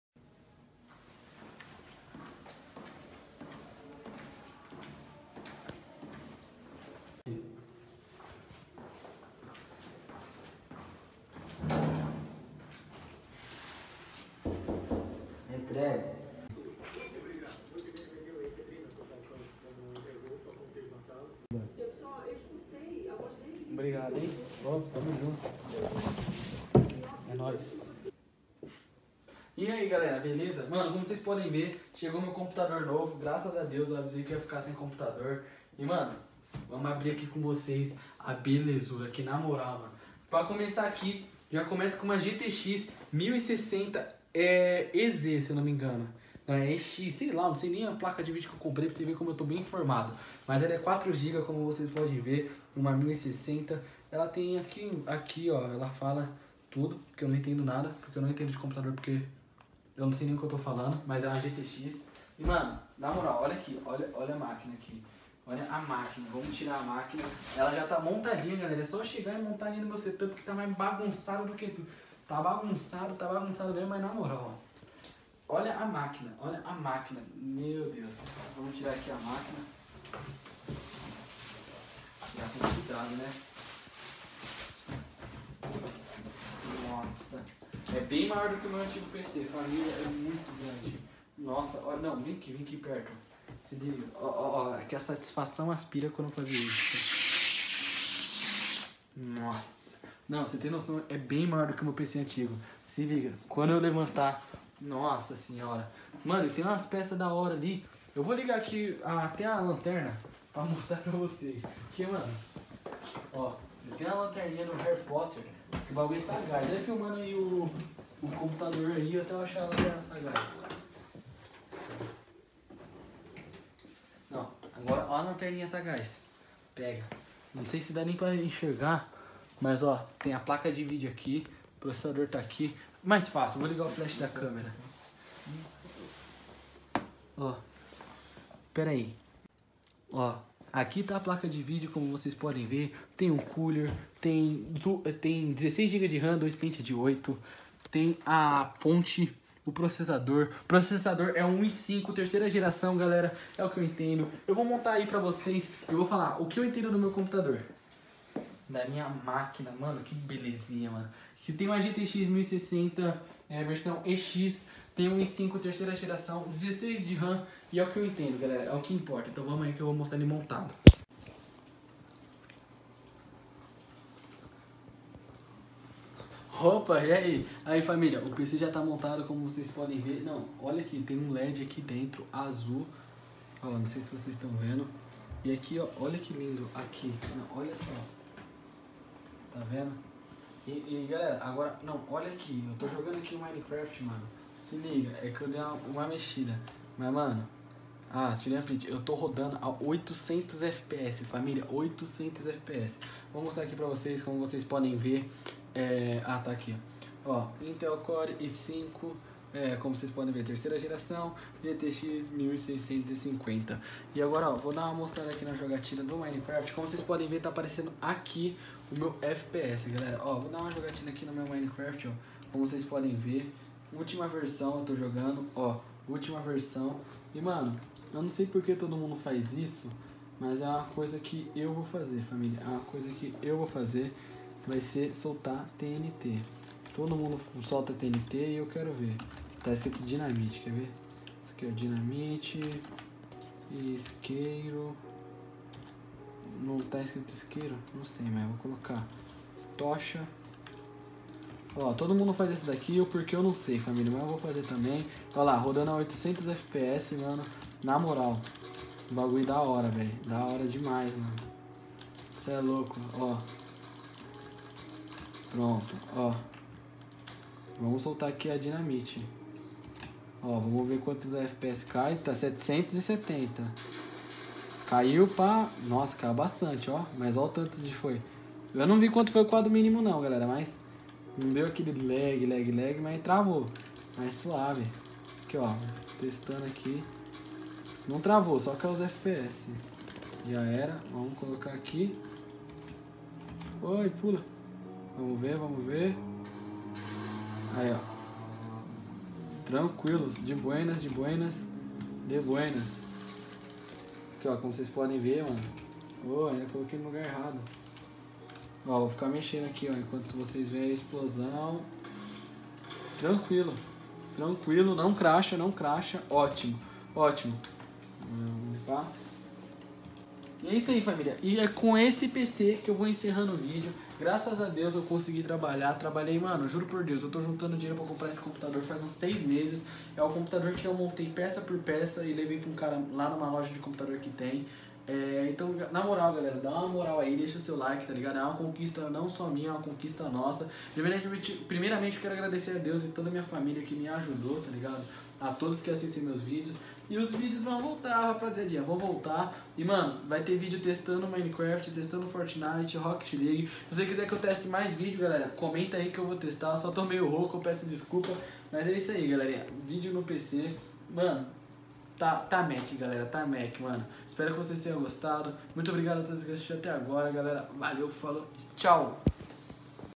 MEU NOVO PC GAMER!!!
3 vídeos por semana

Xbox live:GeovanyCraft
Sype: Crazy1007

ESPECIFICAÇÃO DO PC
16 GB DE RAM
PROCESSADOR: I5 3200
PLACA DE VÍDEO: GEFORCE GTX 1650EX
FONTE 600W